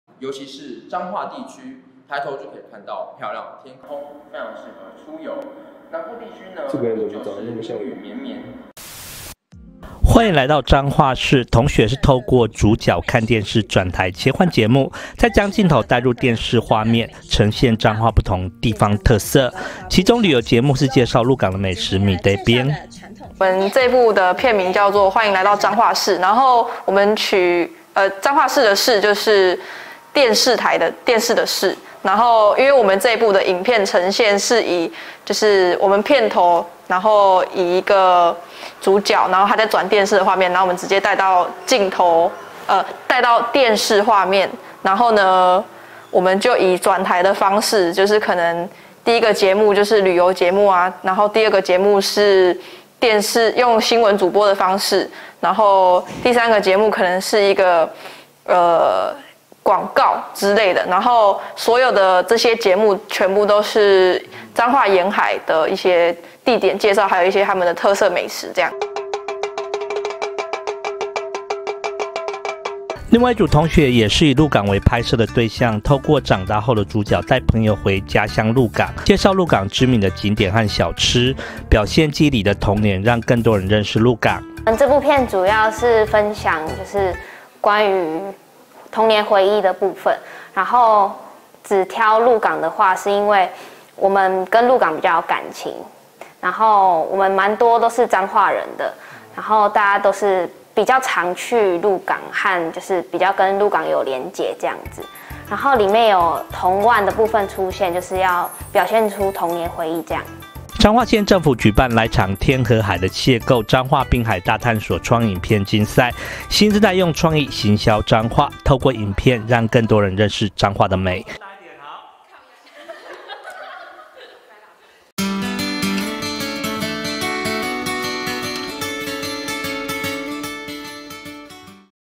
彰化縣政府創意影片競賽 大葉大學傳藝學程獲二佳作
彰化縣政府創意影片競賽 大葉大學傳藝學程獲二佳作 〔記者林明佑／大村鄉報導〕新世代用創意行銷彰化！彰化縣政府舉辦「來場天和海的邂逅－彰化濱海大探索」創意影片競賽，徵求彰化濱海六鄉鎮伸港鄉、線西鄉、福興鄉、鹿港鎮、芳苑鄉、大城鄉主題影片，大葉大學傳播藝術學士學位學程三年級學生拍攝「歡迎來到彰化視」與「我𤆬你來鹿港」，兩組作品都獲頒佳作。@changhuanews